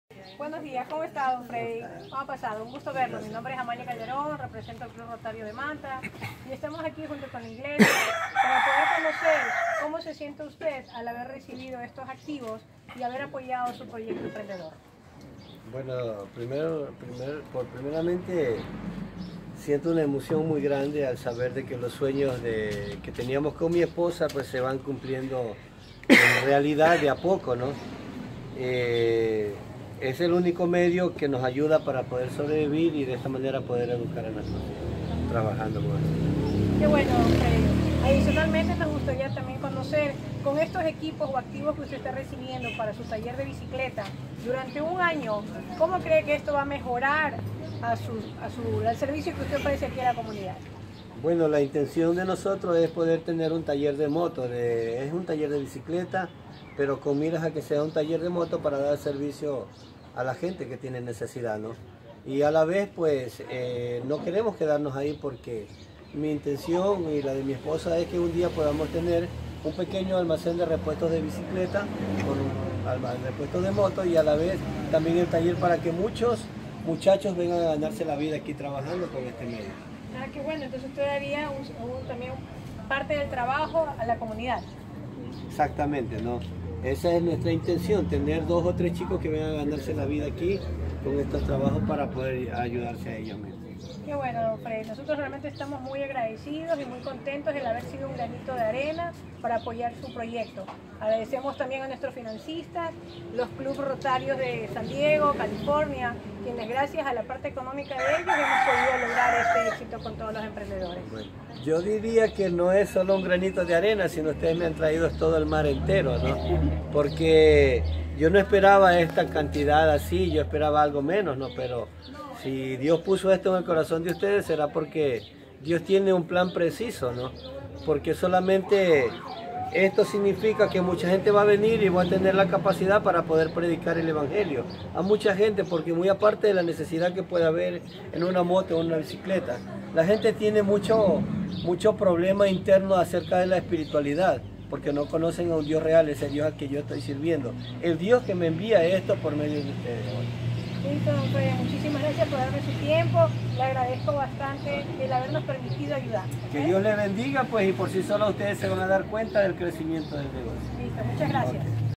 Ecuador Grant Beneficiary: Fredy Moreira Testimonial
Fredy began his business of repairing bicycles on the side of the road with the only savings he and his wife had, $35USD. He would make 80 cents to a 1 USD a day. It was always his dream to see his roadside bike tent evolve into a bigger workshop that could service bicycles and motorcycles. With the help from Rotary he was able to secure more tools and grow his business which was impacted by the earthquake. He plans to hire 2-3 workers in the next few months, and is very thankful to God and Rotary for the resources and training he has received in the aftermath.